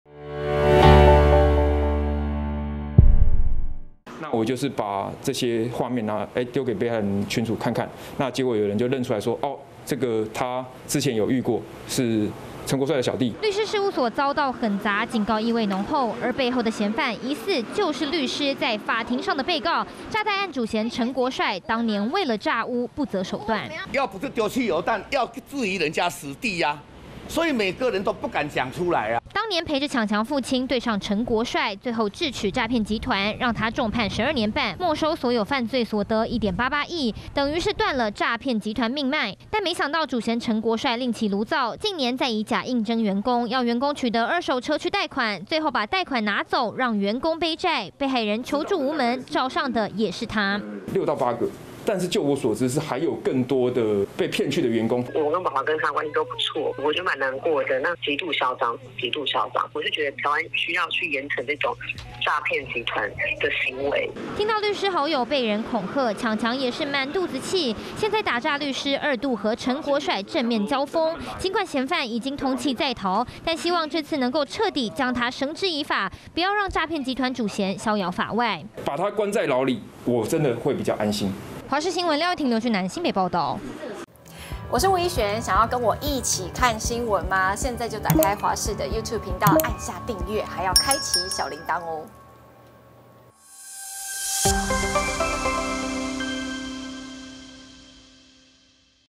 新仇舊恨? 律所疑接手薔薔父詐貸勝訴遭砸｜華視新聞 20221019
而這次律師事務所被狠砸，案情並不單純！因為黃泓勝律師擅長的就是詐騙案，這一次遭針對，他把嫌犯截圖傳進詐騙自救會群組，就有人立刻認出，這兩人就是當年詐貸案主嫌陳國帥的小弟！黃泓勝先前因為薔薔爸爸的詐屋案對上他，這回又幫忙處理車子詐貸案，主嫌也是他，兩人二度狹路相逢，懷疑是因此遭到教訓。